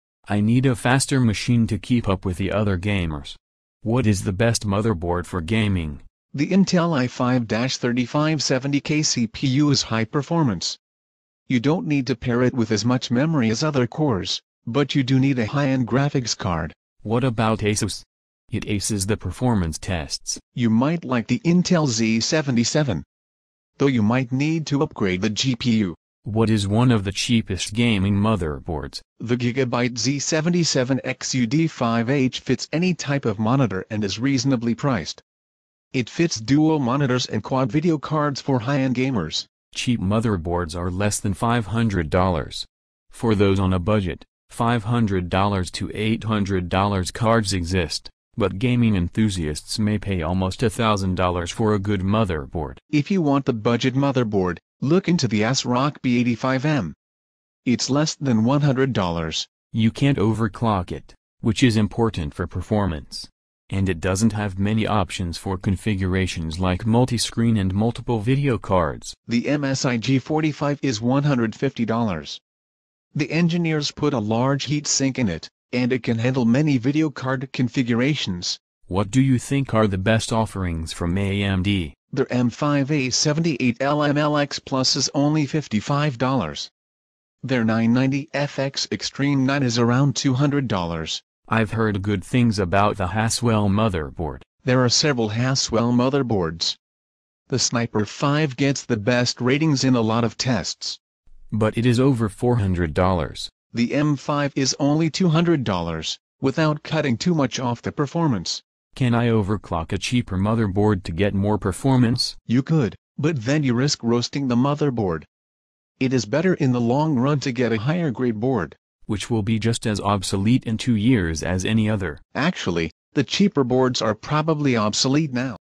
What is the Best Motherboard for Gaming?
What is the best gaming motherboard ever? How to build the ultimate gaming computer!

I need a faster machine to keep up with the other gamers. What is the best motherboard for gaming?

The Intel i5-3570k CPU is high performance. You don't need to pair it with as much memory as other cores, but you do need a high end graphics card.

What about Asus? It aces the performance tests.

You might like the Intel Z77. Though you might need to upgrade the GPU.

What is one of the cheapest gaming motherboard?

The Gigabyte Z77X UD5H fits any type of monitor and is reasonably priced. It fits dual monitors and quad video cards for high end gamers.

Cheap motherboards are less than $500. For those on a budget, $500 to 800 cards exist, but gaming enthusiasts may pay almost a thousand dollars for a good motherboard.

If you want the budget motherboard, look into the ASRock B85M. It's less than $100.

You can't over-clock it, which is important for performance. And it doesn't have many options for configurations like multi-screen and multiple video cards.

The MSI G45 is $150. The engineers put a large heat sink in it, and it can handle many video card configurations.

What do you think are the best offerings from AMD?

Their M5A78L-M LX Plus is only $55. Their 990FX Extreme9 is around $200.

I've heard good things about the Haswell motherboard.

There are several Haswell motherboards. The Sniper 5 gets the best ratings in a lot of tests.

But it is over $400 dollars.

The M5 is only $200, without cutting too much off the performance.

Can I overclock a cheaper motherboard to get more performance?

You could, but then you risk roasting the motherboard. It is better long run to get a higher grade board.

Which will be just as obsolete in two years as any other.

Actually, the cheaper boards are probably obsolete now.